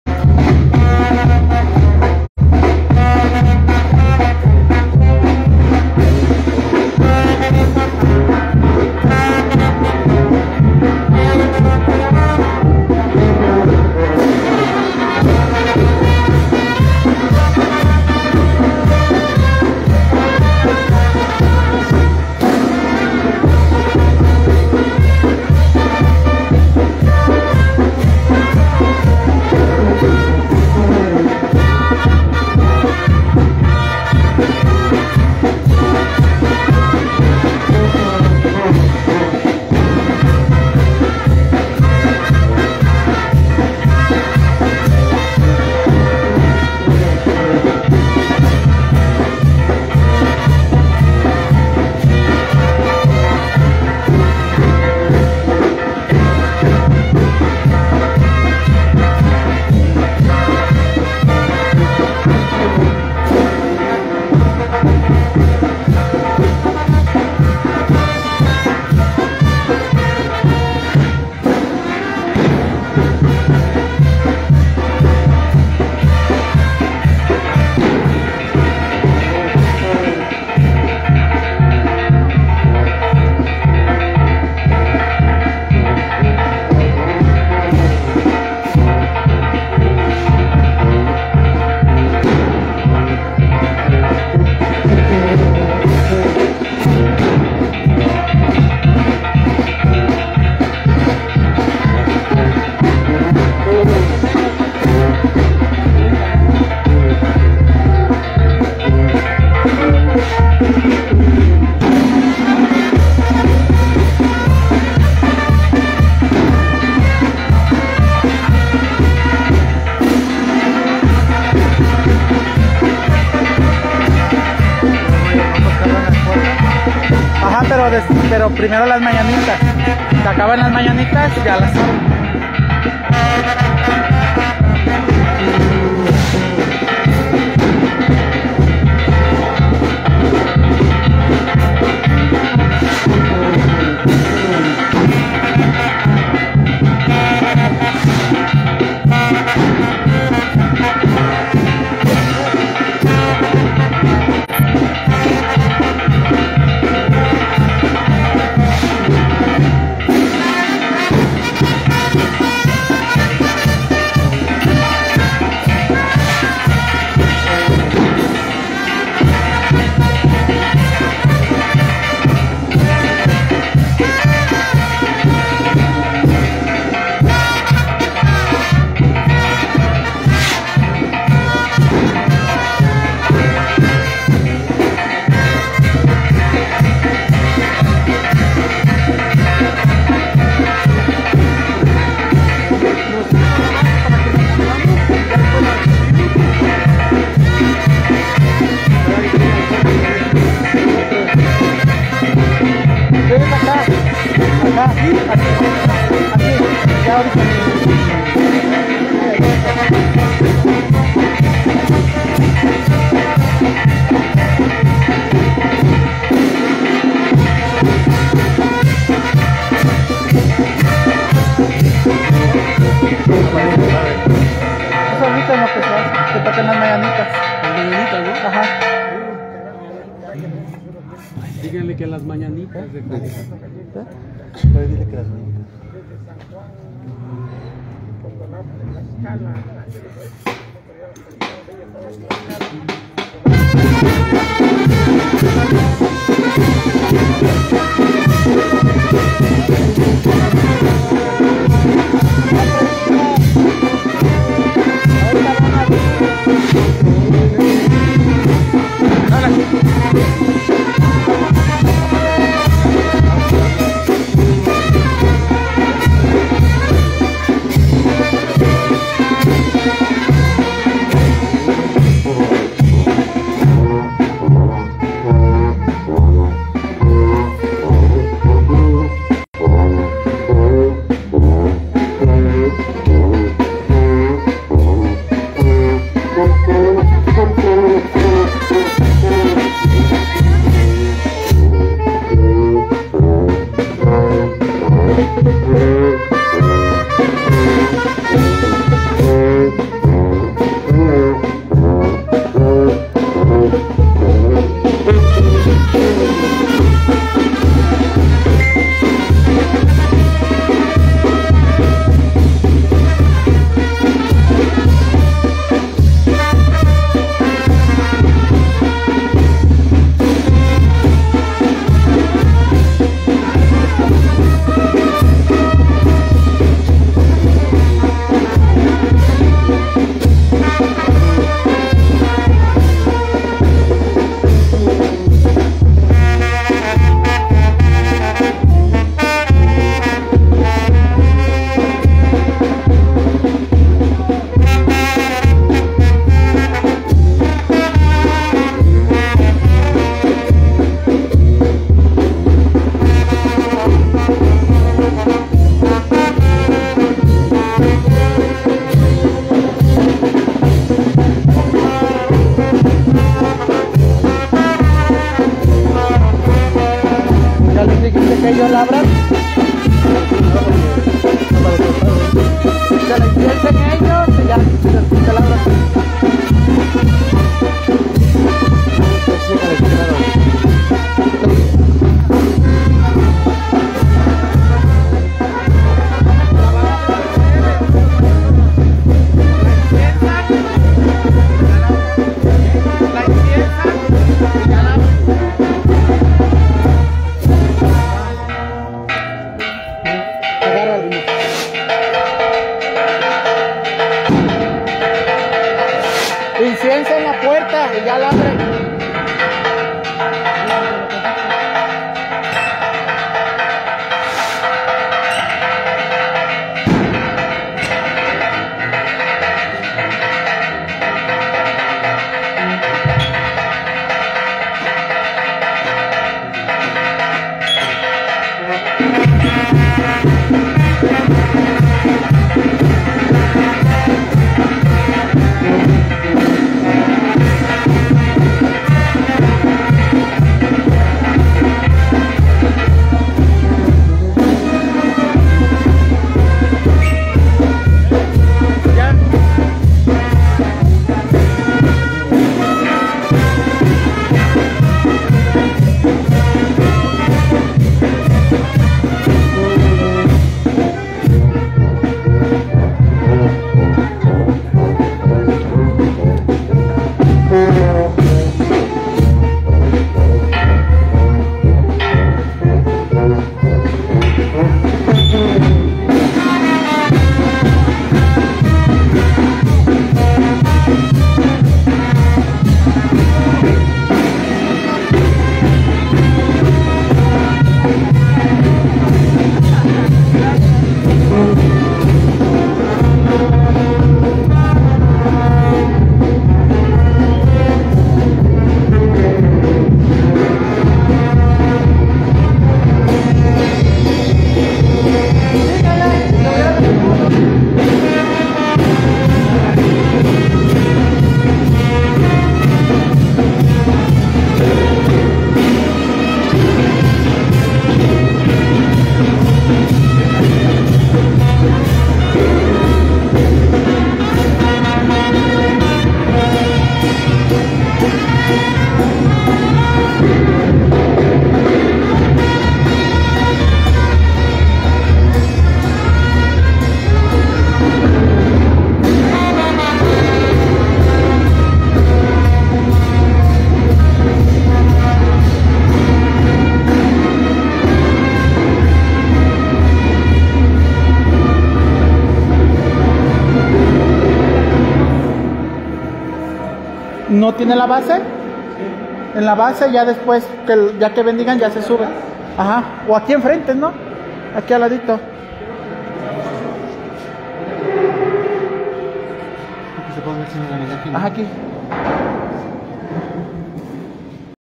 Procesión de las imágenes junto con la imagen patronal del barrio de la santa cruz para la gran reap
Procesión de las imágenes junto con la imagen patronal del barrio de la santa cruz para la gran reapertura después del sismo del 19